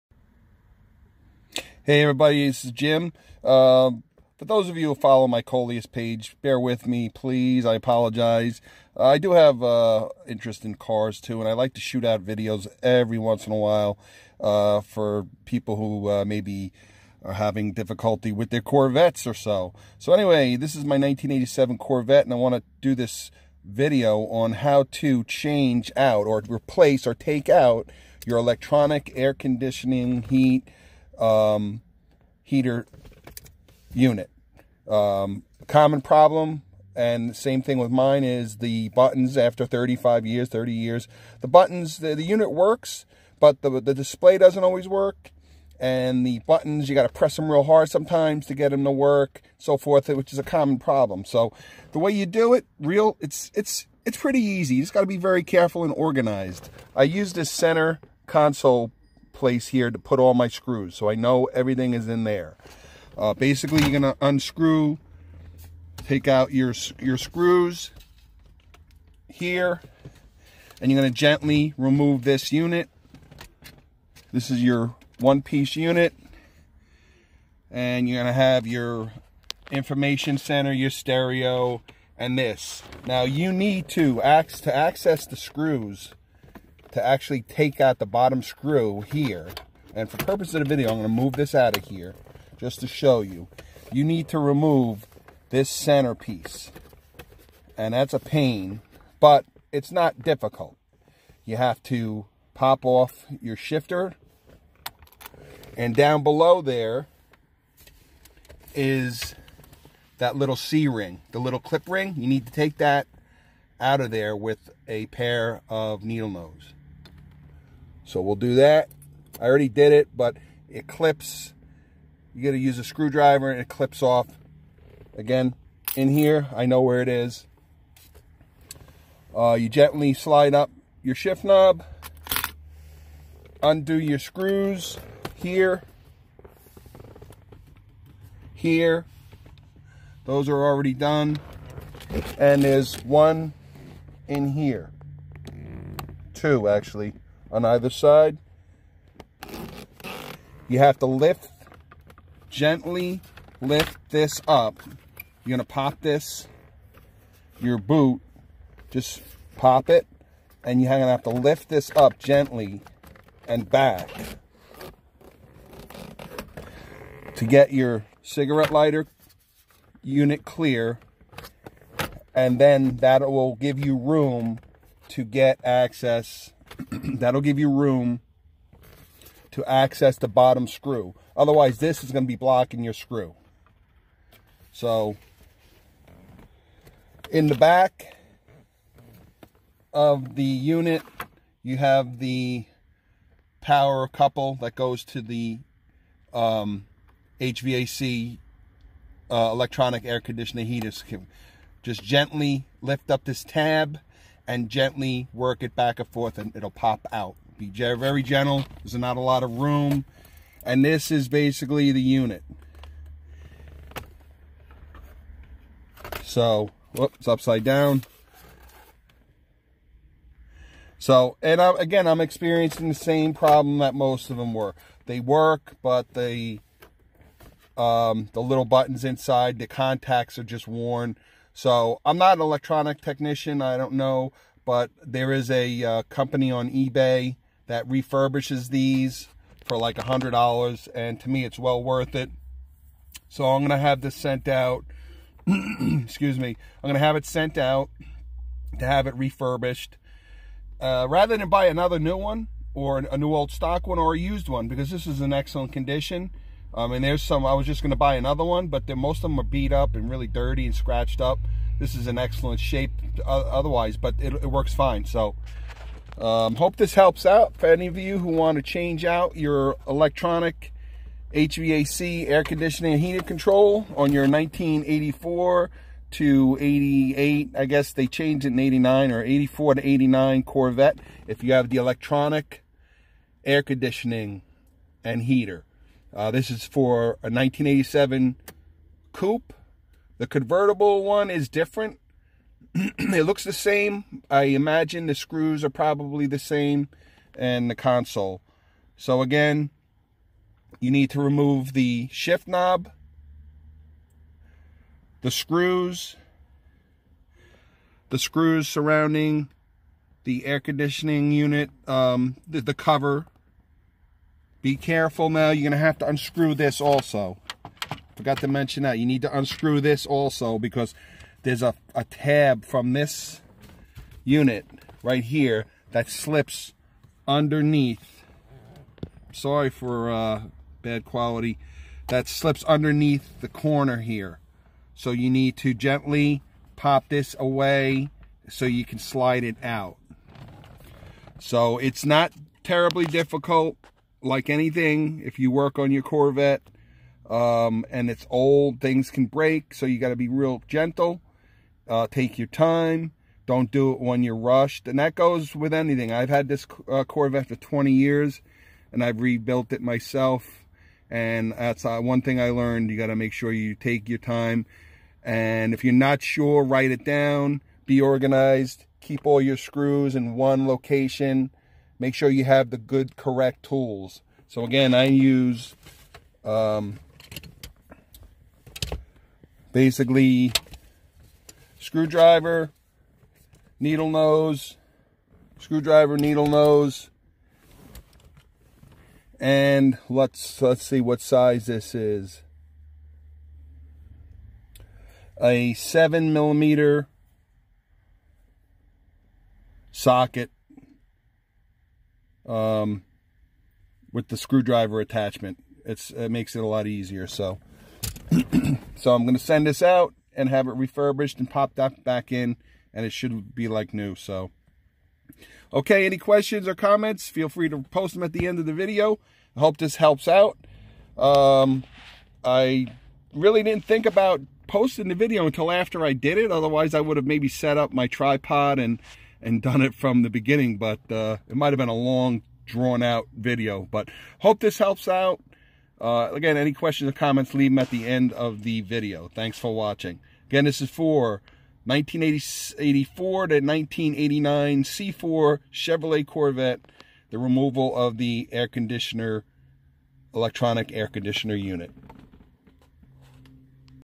C4 CORVETTE OWNERS WATCH THIS!! Before You buy a new Electronic Climate Control AC Heat Control!
Here I explain how to simply remove your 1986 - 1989 Chevy Corvettes Electronic climate control A/C,  Heat & Defroster unit out  from your Dashboard to have it repaired.
  I show you what screws to remove , some tricky parts of the dash covers and some potential trouble areas you may run into.
So whether your removing and replacing an old inoperable unit for a new refurb unit or having the new front face plate replaced or repaired. This will make a breeze !! DON'T BUY A NEW UNIT...MOST CAN BE EASILY FIXED .